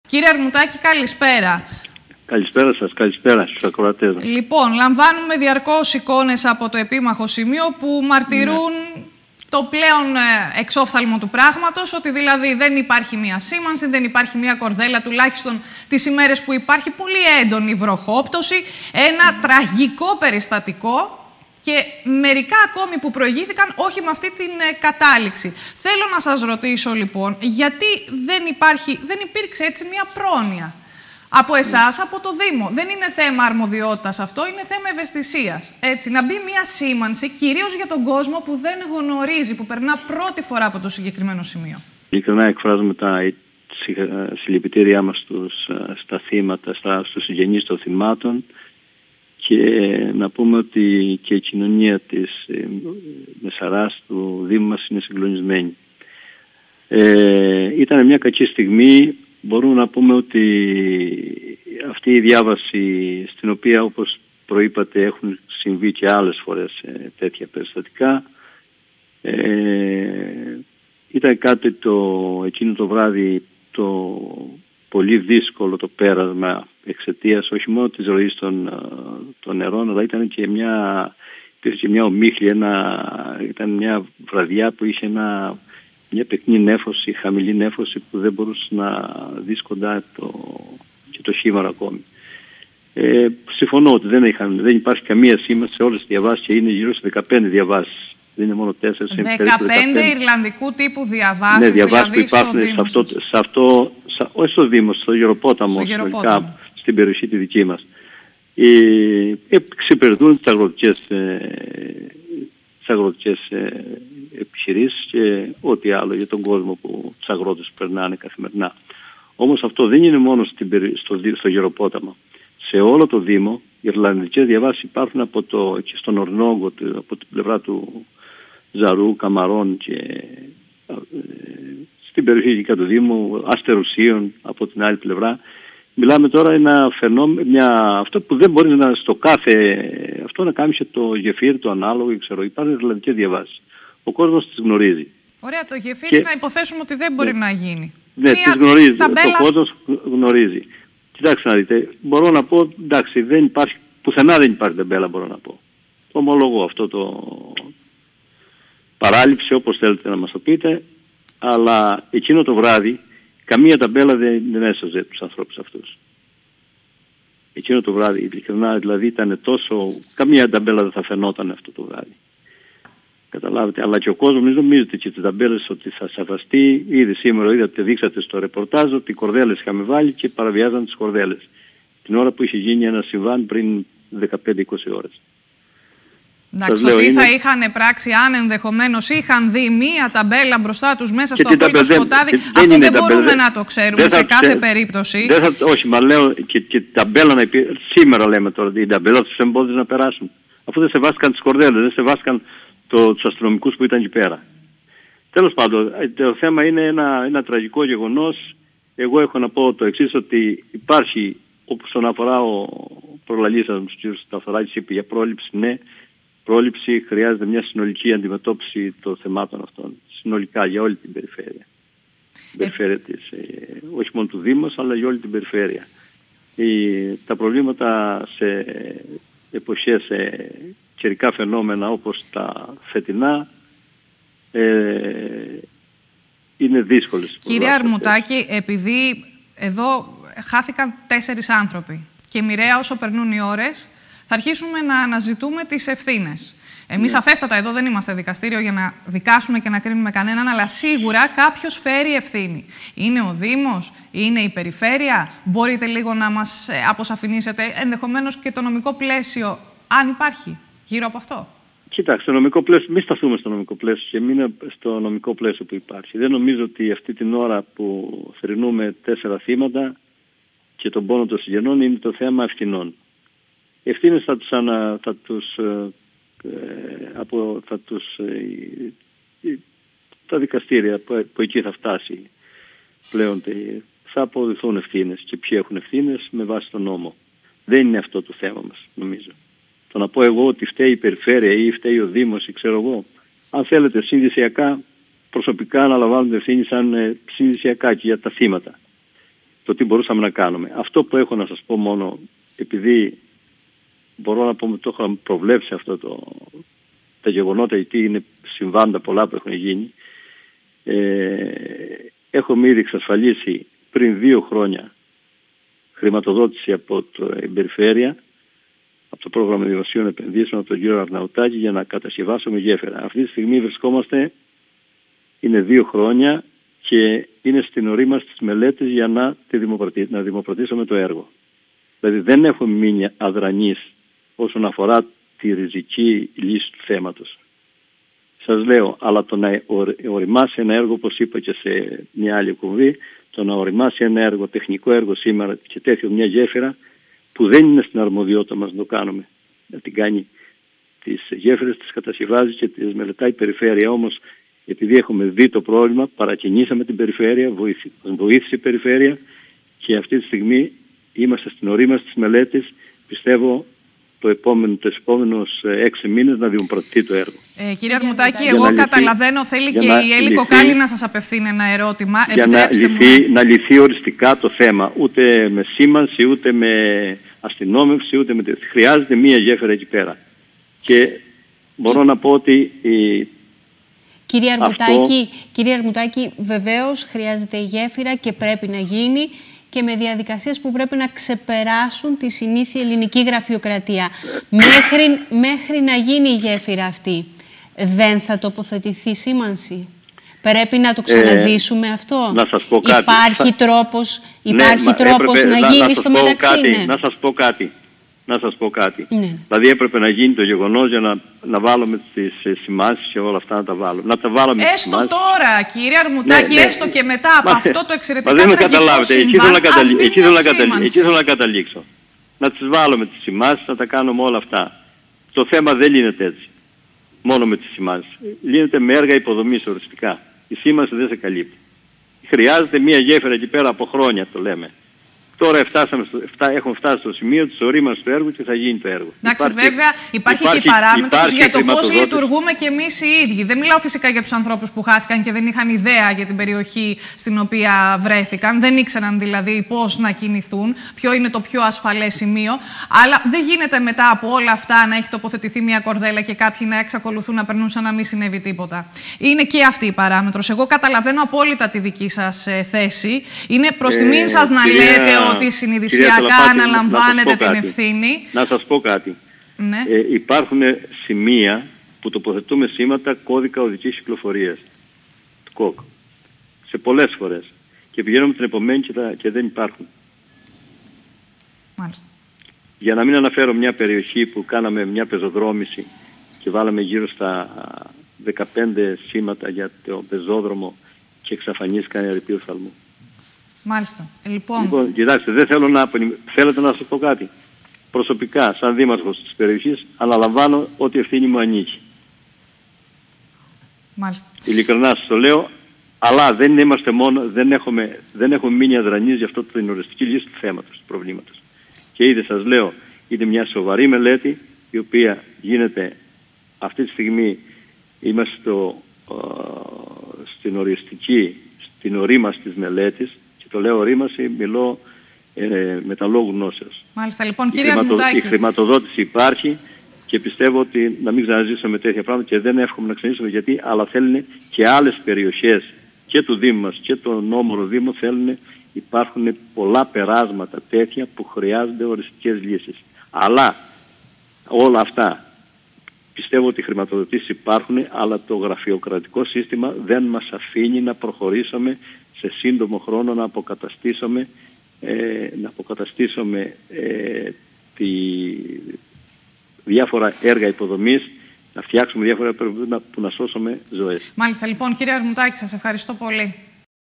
"Συνειδησιακά αναλαμβάνω την ευθύνη"
Για την τραγωδία του Γεροποτάμου που άφησε πίσω της 4 νεκρούς μίλησε στο κεντρικό δελτίο ειδήσεων της Kρήτη tv ο δήμαρχος Φαιστού , Γιώργος Αρμουτάκης. Συγκλονισμένος και ο ιδιος δήλωσε "συνειδησιακά αναλαμβάνω την ευθύνη" ενω τόνισε οτι δεν πρόκειται για το μοναδικό φονικό σημείο.
 Συνολικά στην ευρύτερη περιοχή του Γεροποτάμου υπάρχουν 15 ακόμη διαβάσεις "ιρλανδικού" τύπου.
Δείτε τα όσα χαρακτηριστικά είπε.
Στο στούντιο η δημοσιογράφος Κατερίνα Σαλαπάτα